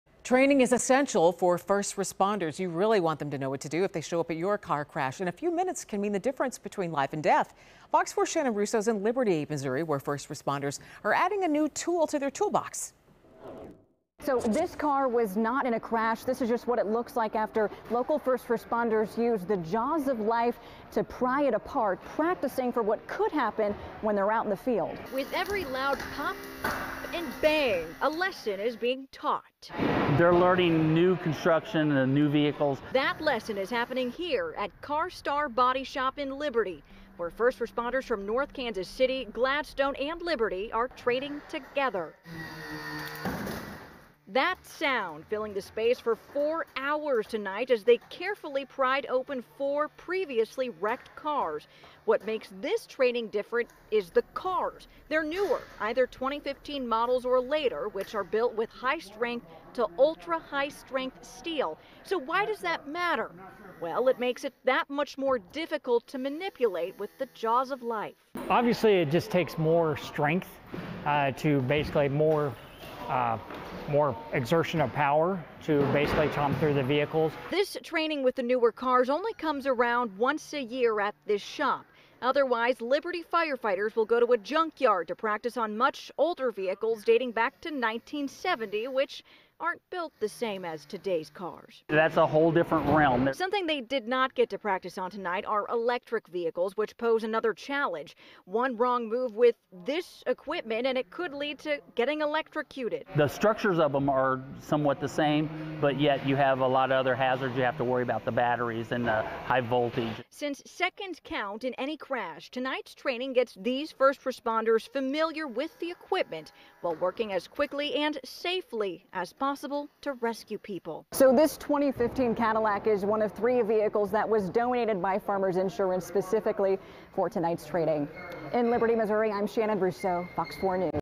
Kansas City-area first-responders train for dangerous wrecks
First responders in Kansas City's Northland are brushing up on their training when it comes to rescuing people from dangerous car crashes.